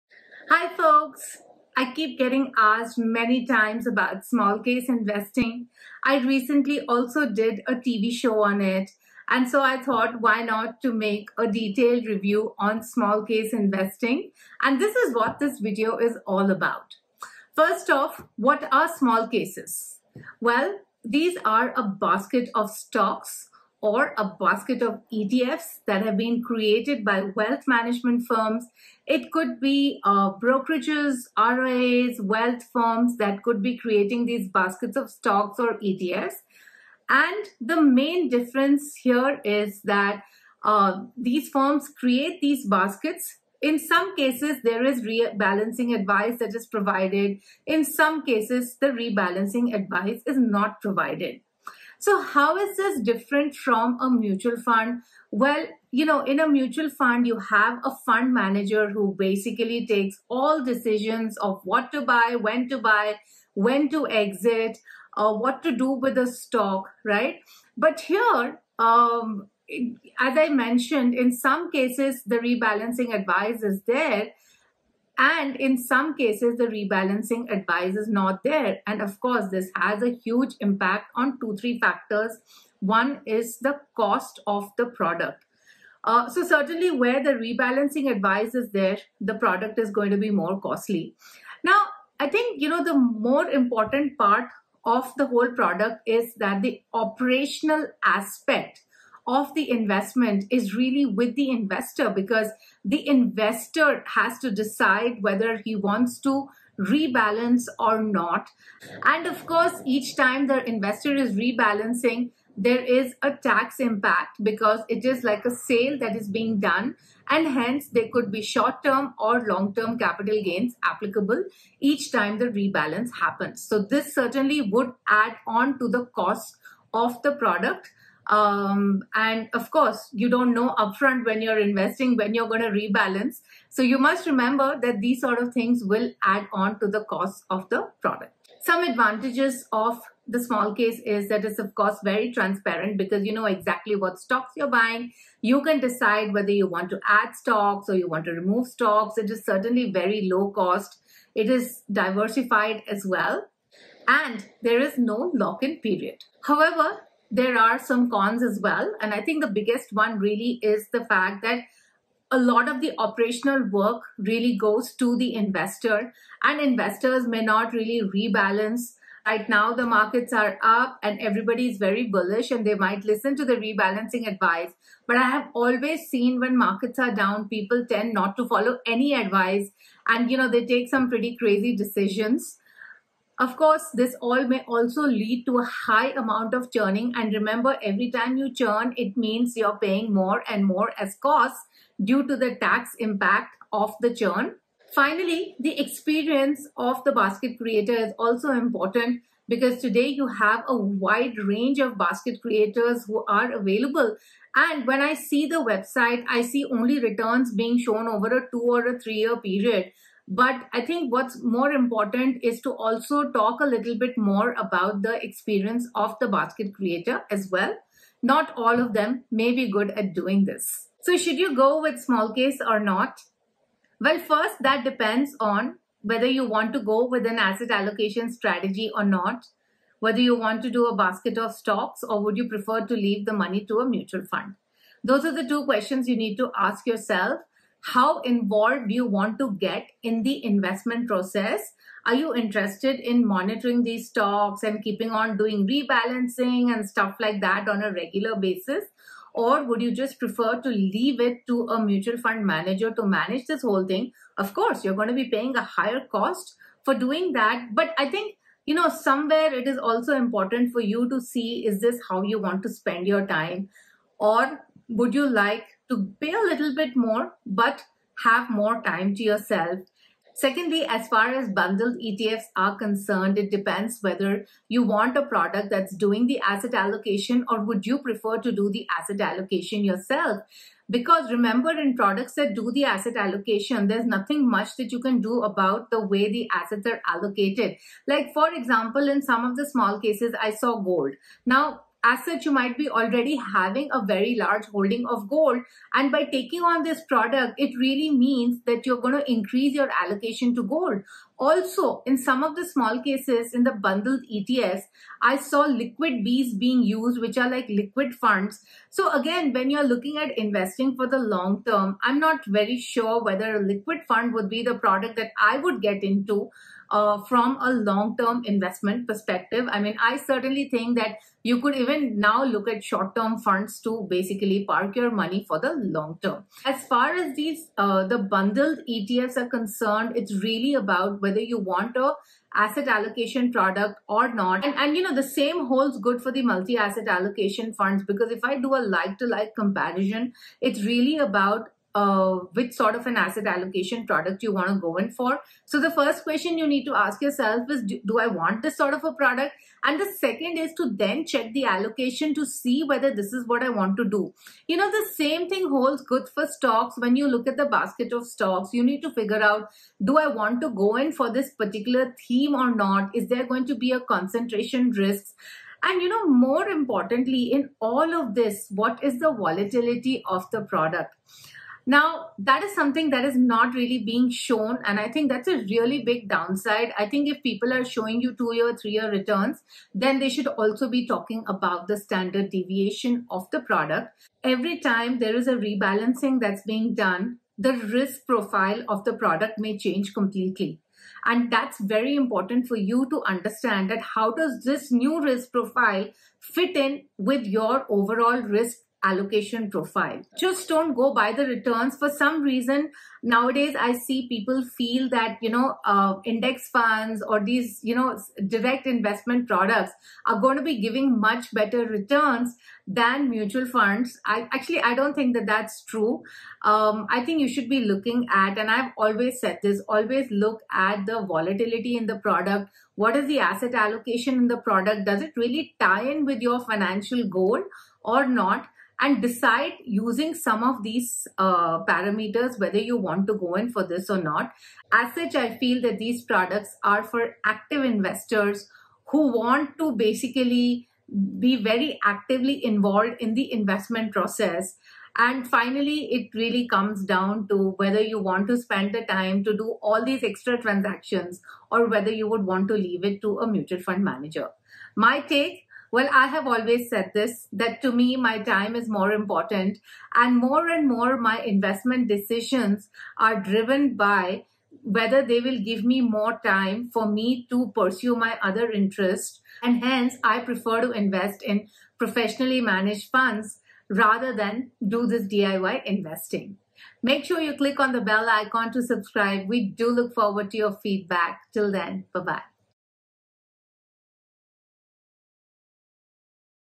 Smallcase Review
What are small cases? How do they work? How are they different from mutual funds? Which should one pick - small cases or mutual funds? Watch the video to know more.

Contents of this video:

00.00 - Introduction
00.18 - What are smallcases?
00.52 - How are smallcases different from mutual funds?
02.18 - Advantages of smallcases
02.36 - Disadvantages of smallcases
03.41 - Should you invest in smallcase?